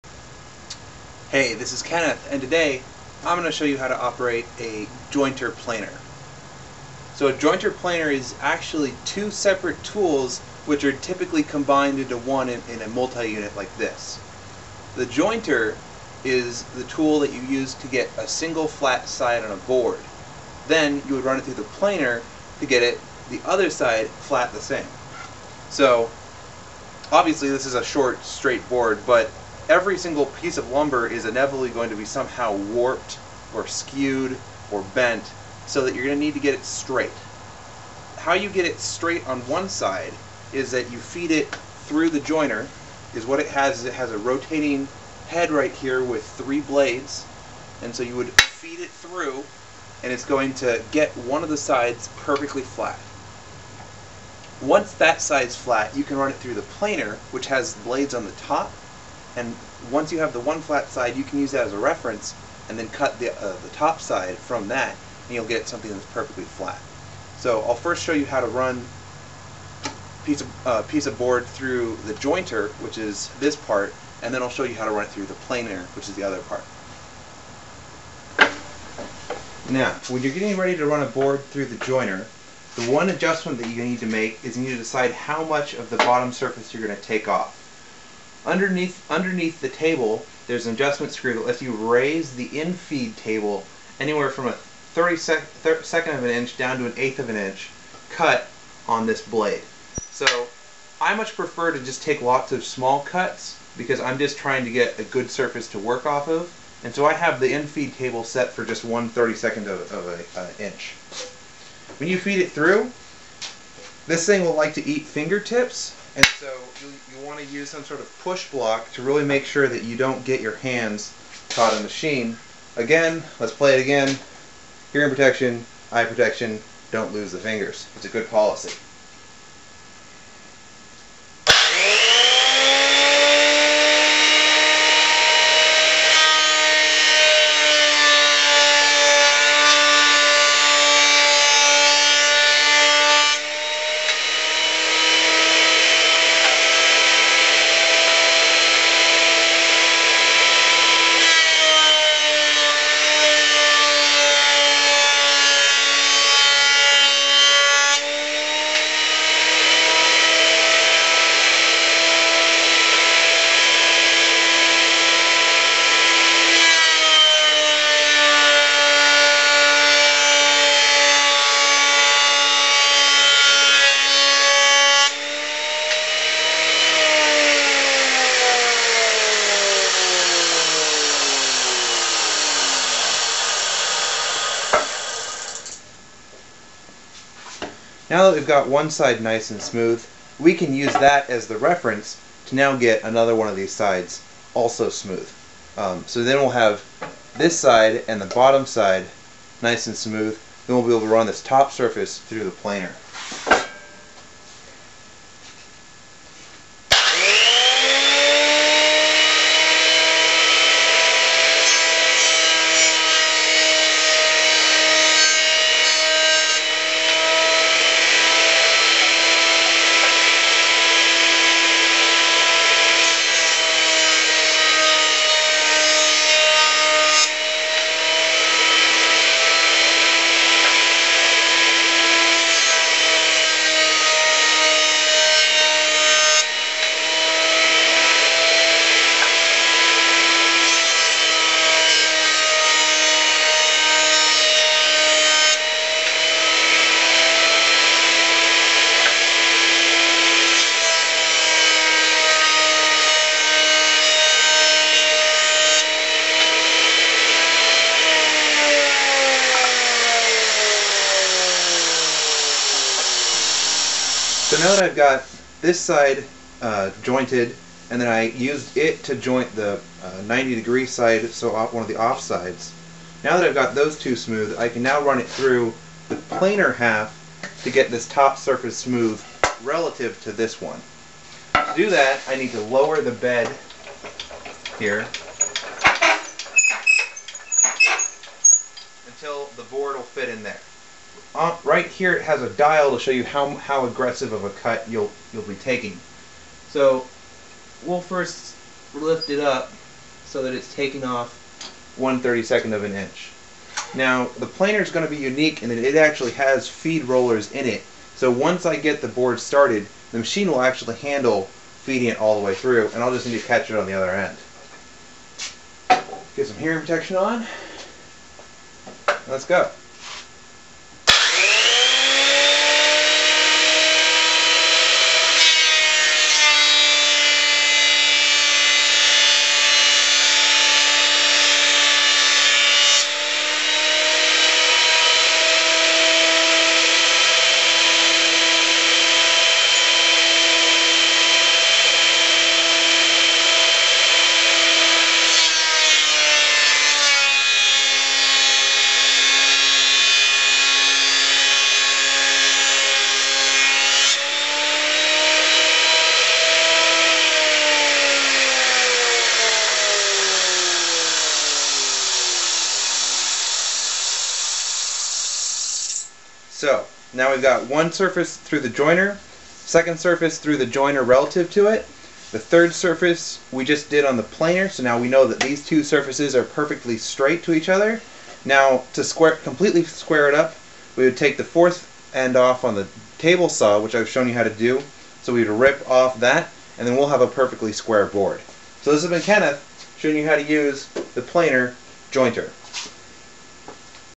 Intro to Planer/Jointer
Basic intro to  how to square boards on a planer / jointer